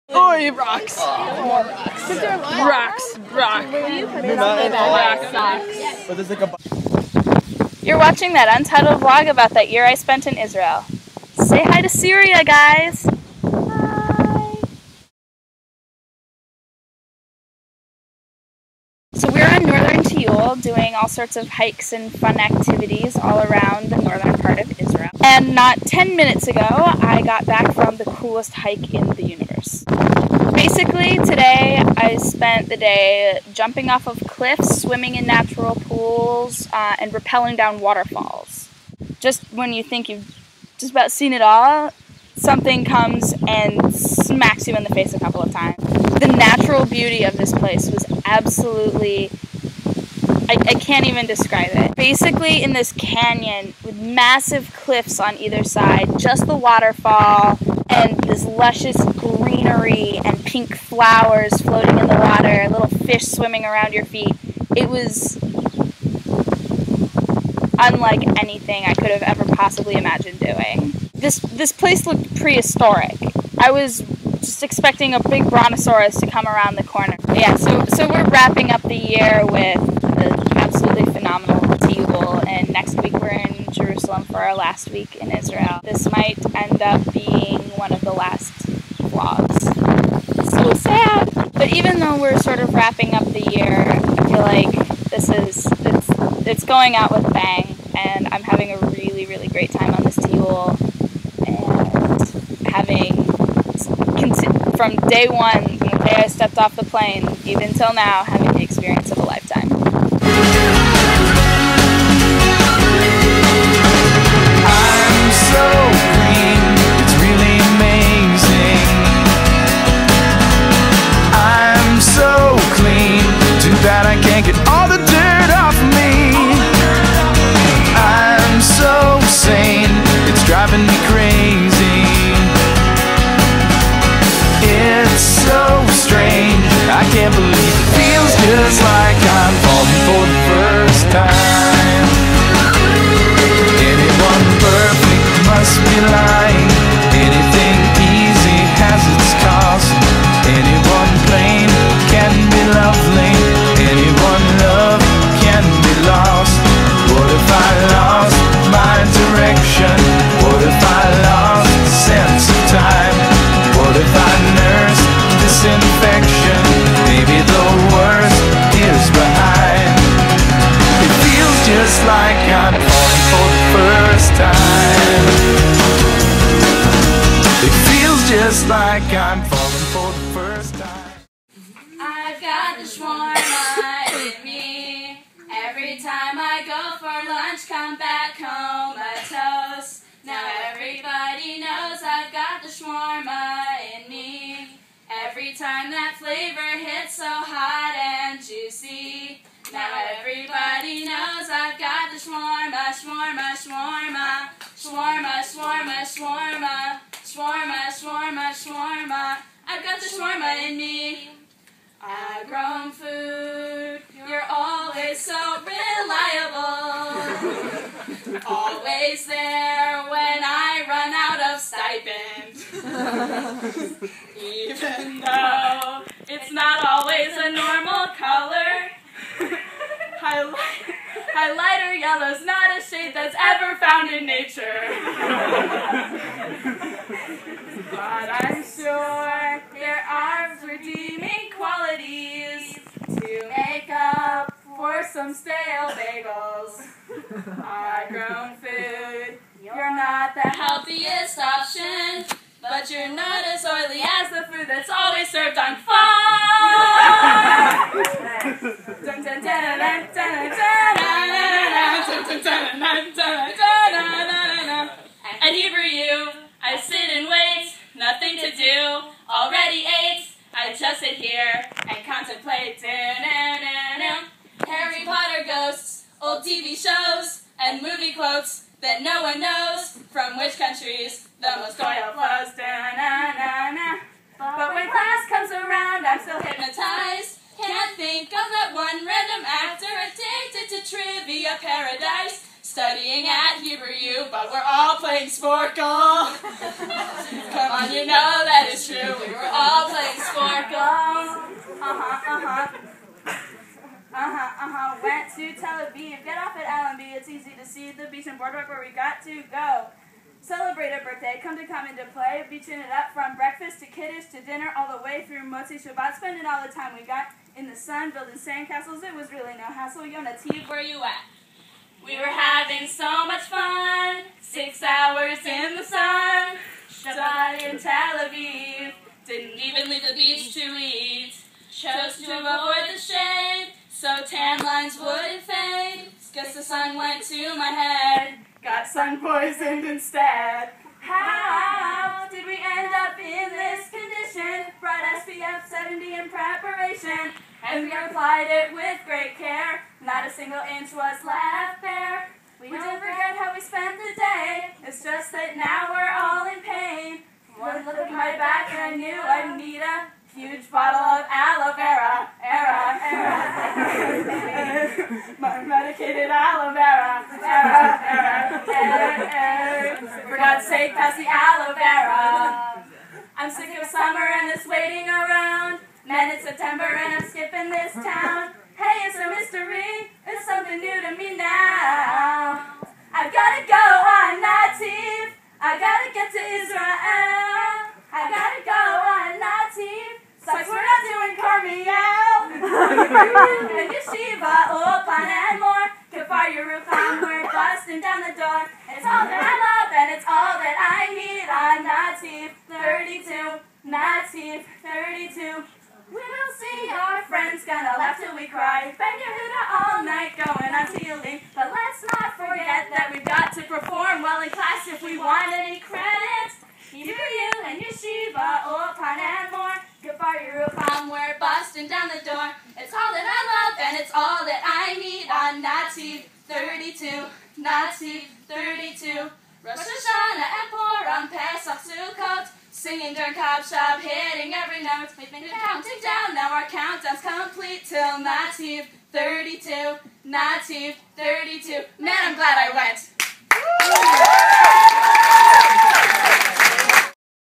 Nativ 32: I Jumped Off a Cliff Today! (Northern Tiyul & the last week)
Apologies for the delay, but here is the final installment of That Untitled Vlog About That Year I Spent in Israel